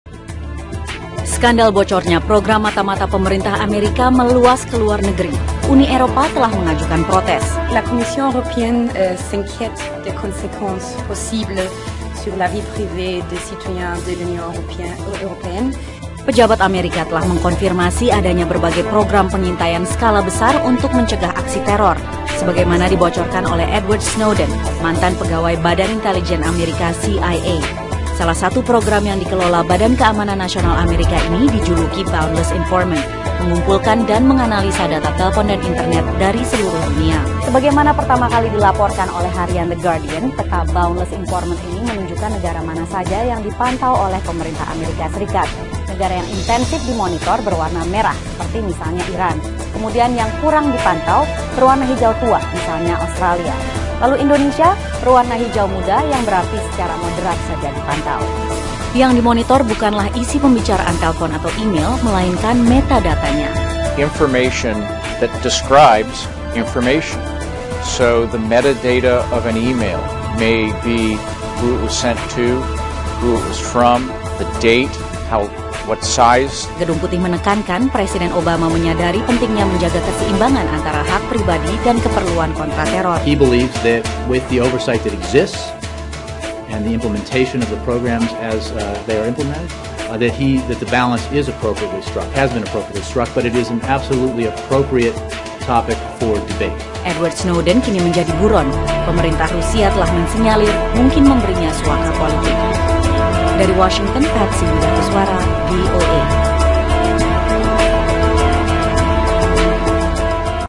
Awas, Indonesia Dimata-matai Amerika
Awas Indonesia rupanya juga dimata-matai Amerika melalui jaringan komunikasi maya di Internet. Jangan asal dalam berkomunikasi di Internet.